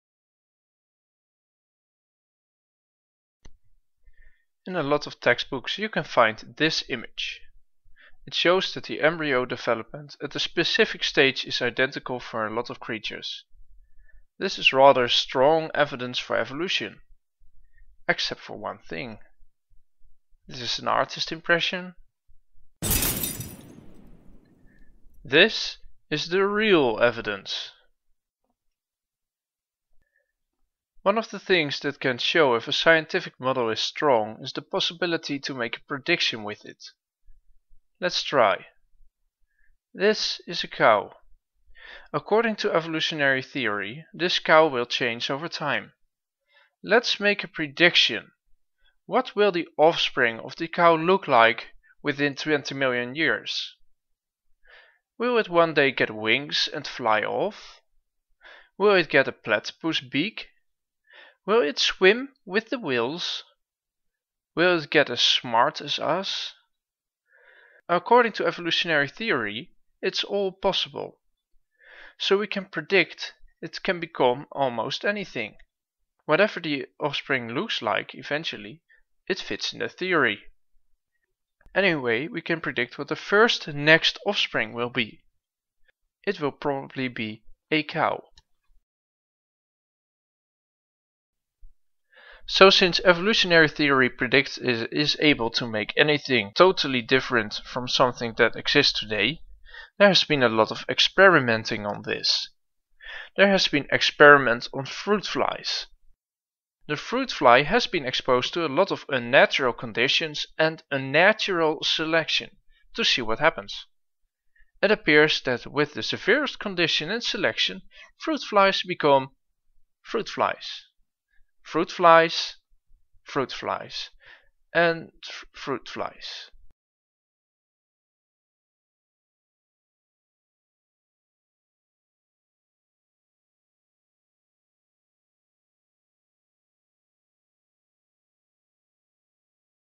Evolution: The Evidence - Episode 13: Embryos, Predictions and Experiments
3 subjects this episode: Embryos, Predictions and Experiments. Do experiments prove evolution? And does evoluntionary theory make clear predictions?